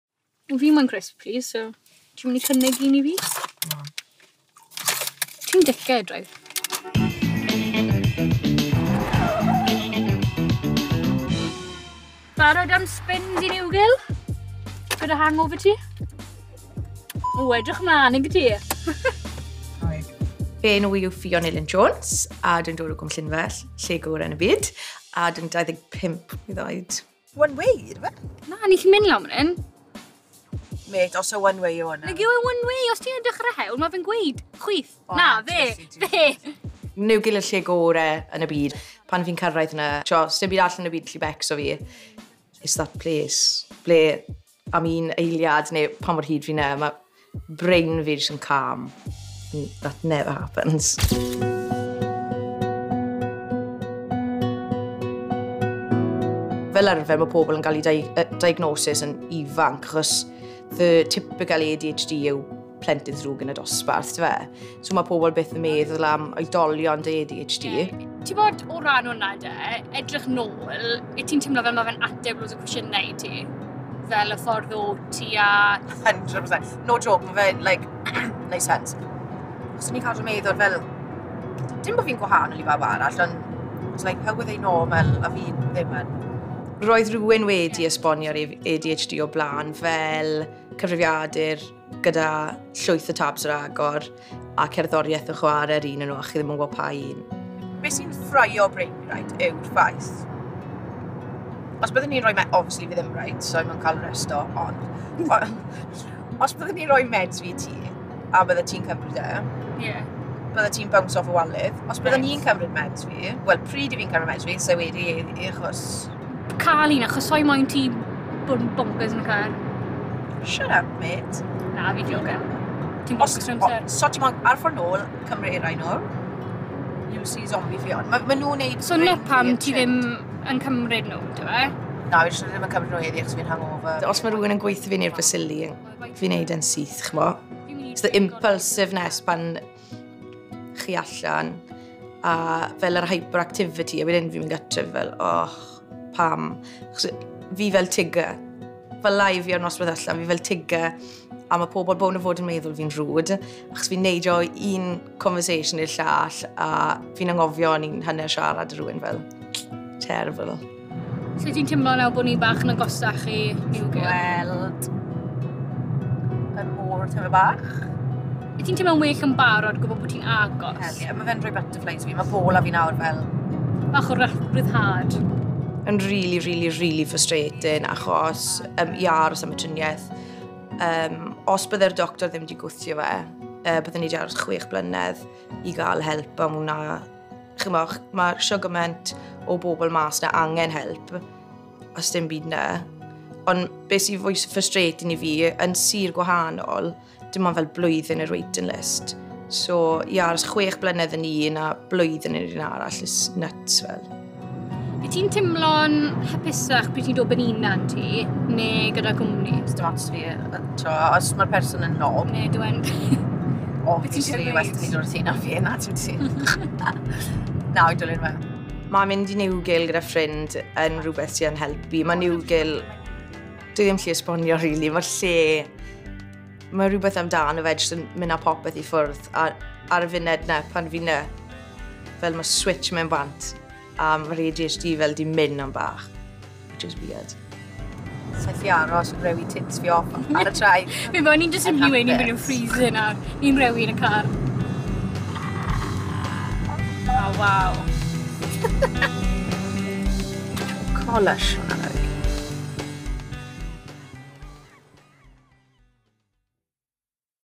Ffion - Y Siwrnai
UN CAR🚗UN SIWRNAI
Mae Ffion Jones ar ei ffordd i i ddihangfa arbennig.